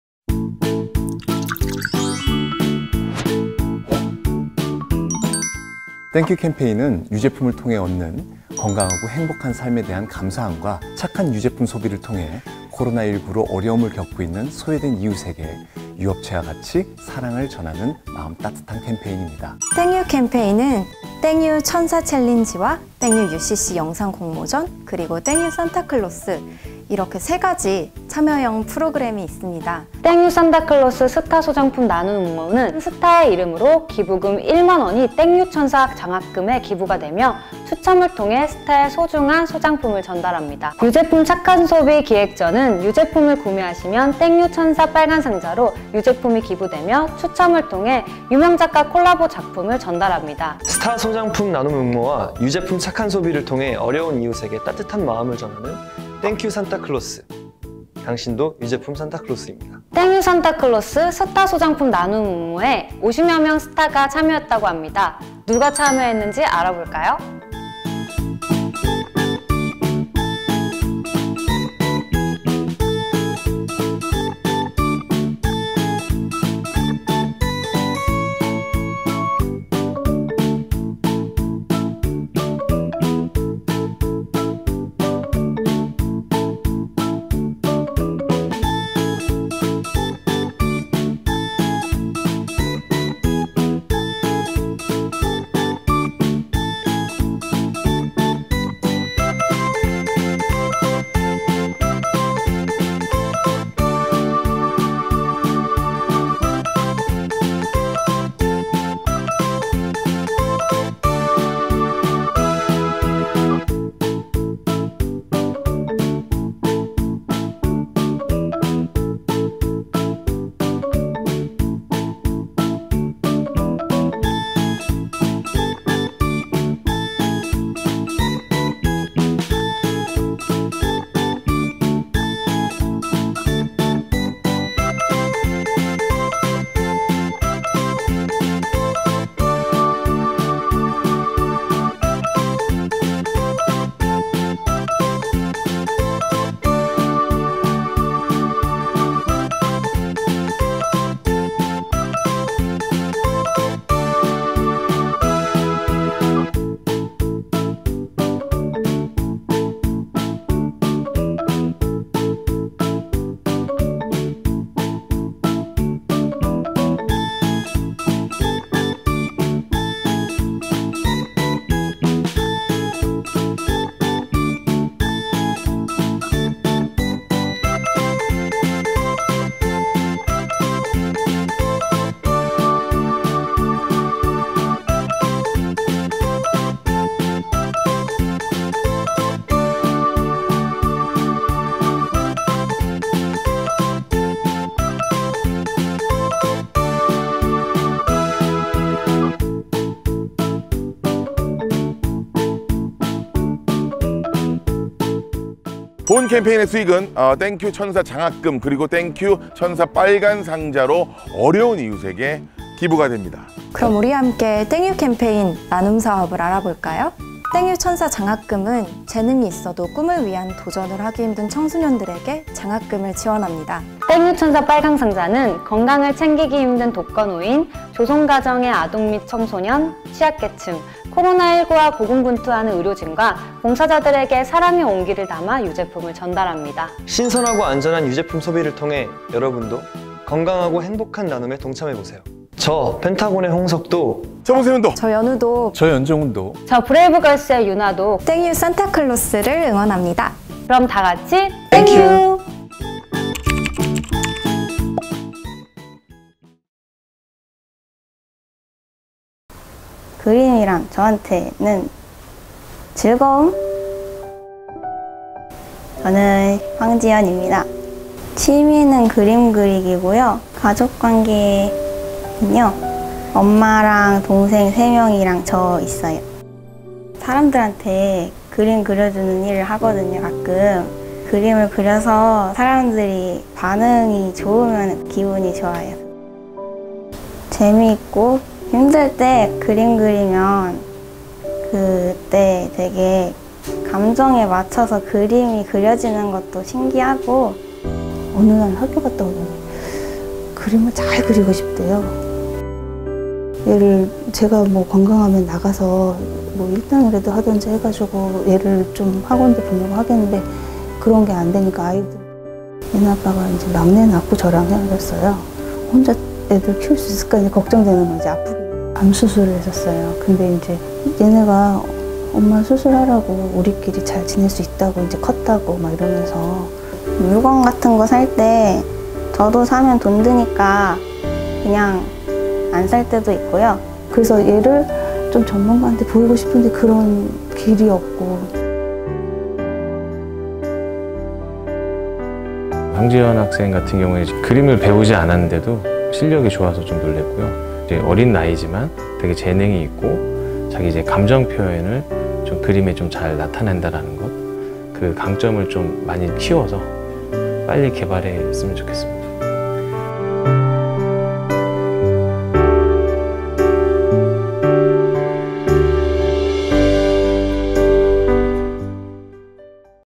[적십자x한국유가공협회] 지현이의 꿈을 응원해주세요, Thank U 캠페인 (feat. 스타★소장품)
*지현이의 꿈을 위해 Thank유캠페인에 참여하고 스타소장품을 만나보세요*

코로나19 등 여러 어려움에 처한 우리나라 유가공 산업도 살리고!
소외된 이웃들에게 따뜻한 마음도 전하는!
일석이조 - "Thank유 캠페인"
신선하고 안전한 유제품 소비로 유업체와 어려운 이웃들을 도와주세요!
여러분이 바로 ☜유제품 1004☞ 입니다♡